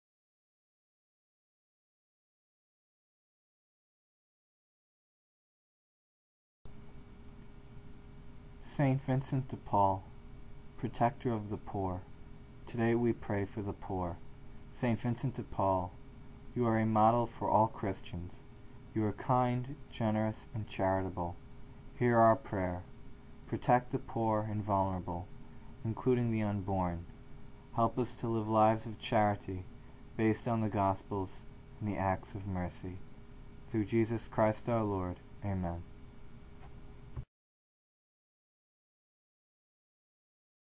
Saint Vincent de Paul (September 27)
September 27 is the feast day of Saint Vincnet de Paul. This prayer is for the poor.